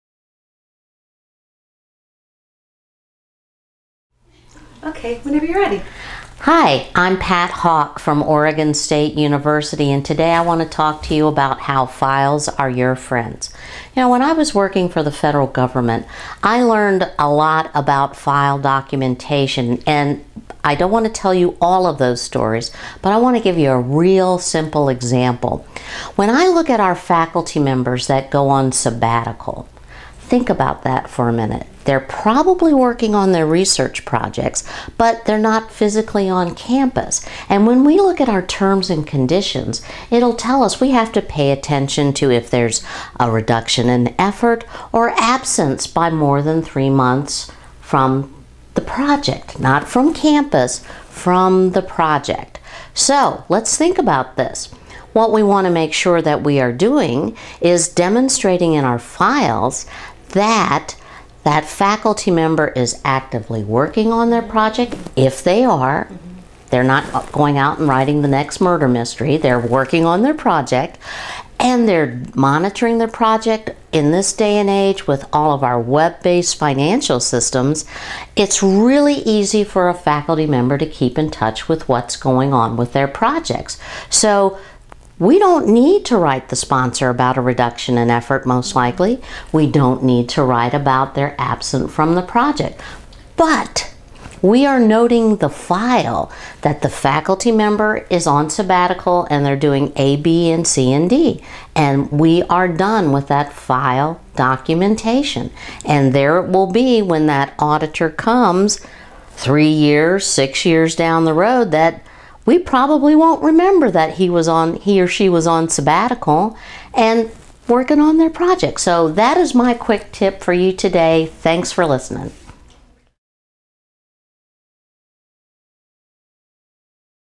Importance of File Documentation
Pat Hawk, Oregon State University, describes the importance of file documentation.